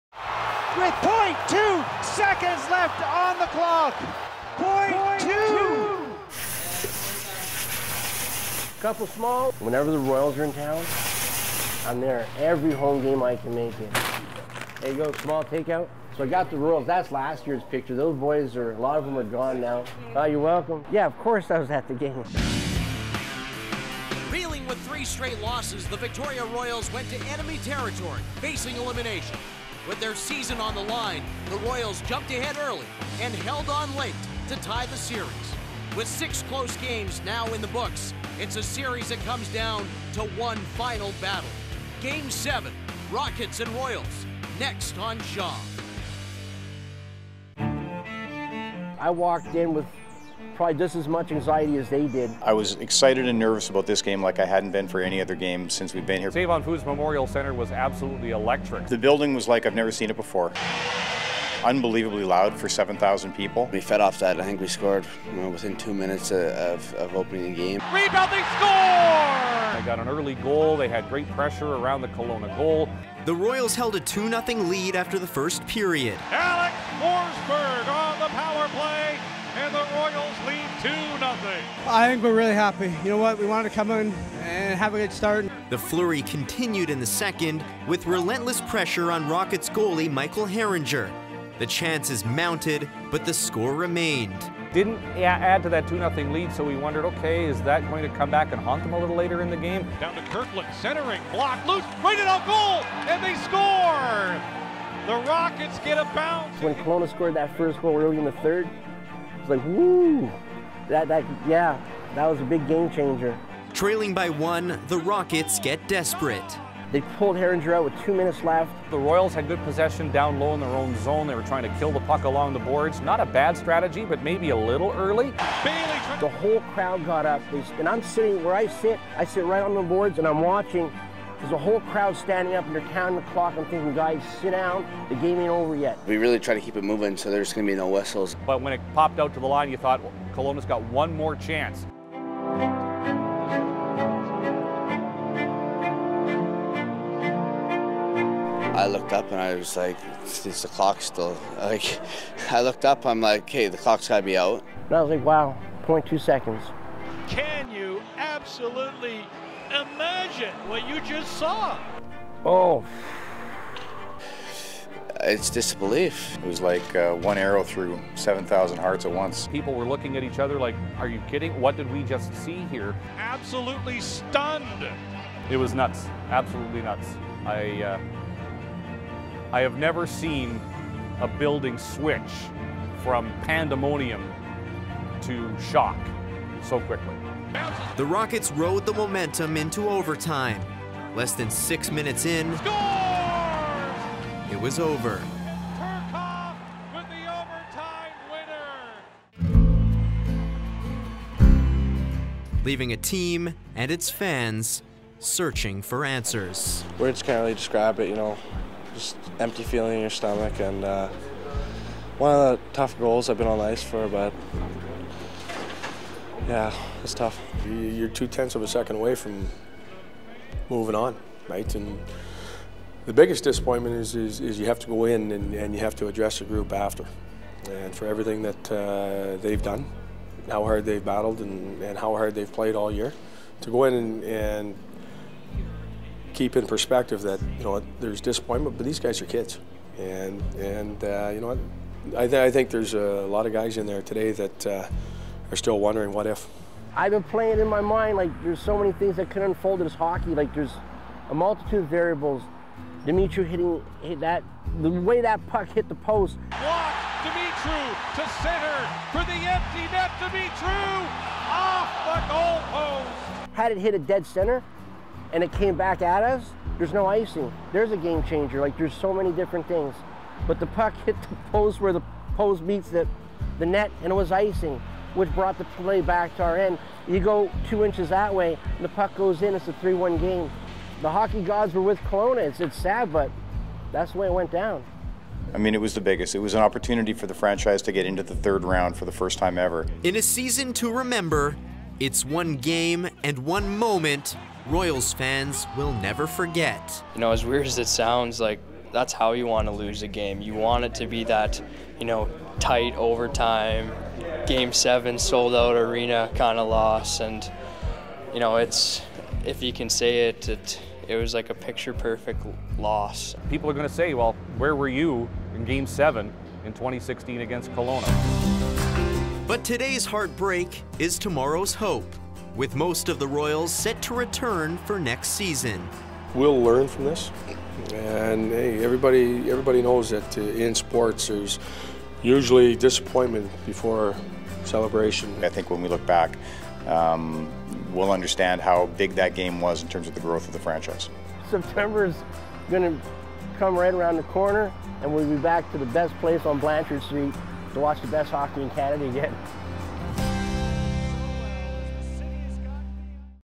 0.2
Shaw TV recaps a WHL Classic: Game 7 - Victoria Royals vs. Kelowna Rockets.  Players, fans, broadcasters and coaches reflect on the wildest game in Victoria Royals history.